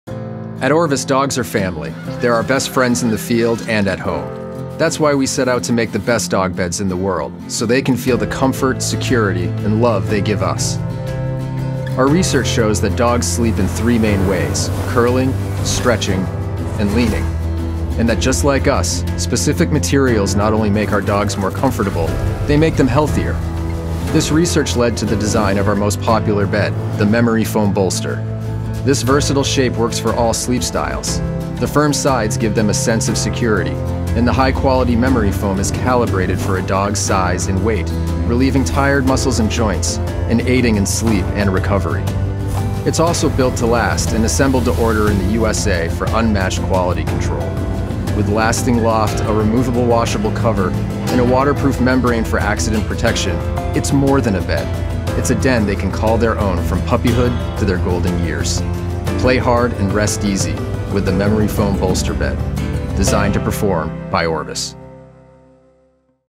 The Orvis Memory Foam Dog Bed – Canine Rest & Recovery
Discover the top dog bed on the market - the Orvis memory foam dog bed. Designed to satisfy all sleep styles, this bed provides the ultimate den setting to support your dog’s rest and recovery. Built to last with human-grade upholstery covers that are machine washable, and highest-quality memory foam insert.